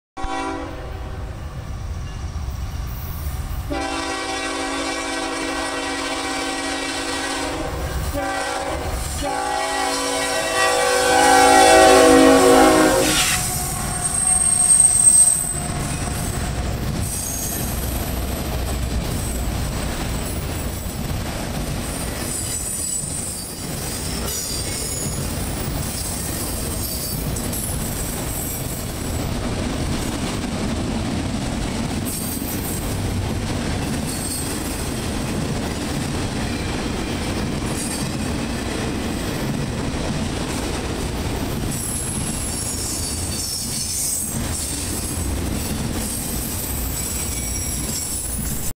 Q217 7/13/2019
Q217 westbound thru point of rocks on 7/13/2019